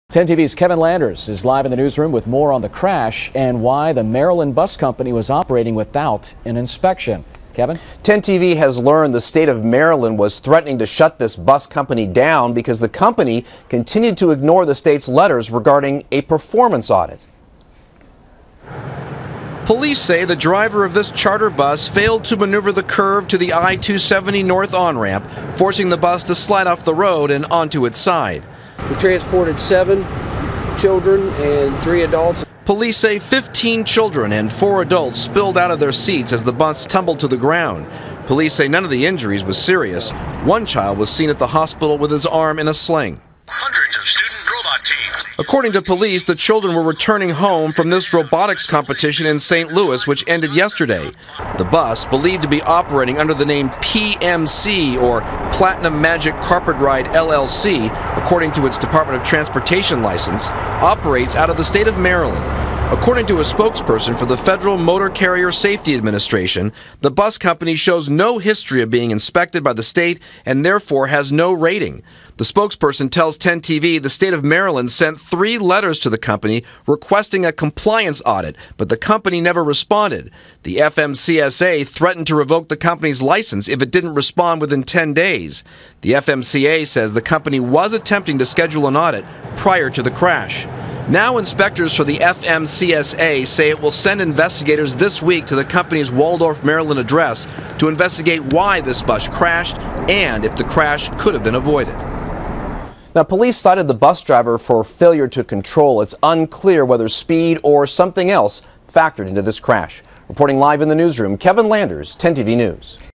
Charter Bus Crash
New information about the bus company involved in a rollover crash